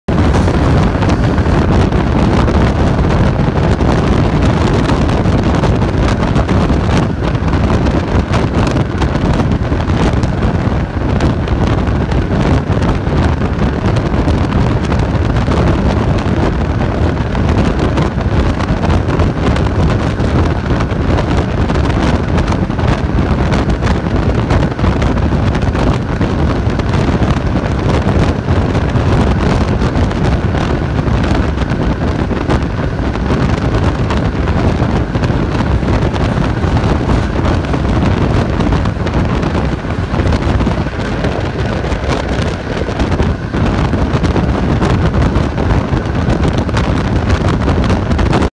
IMG 2231 3过黑石铺
5365次绿皮车，京广线，长沙市天心区黑石铺站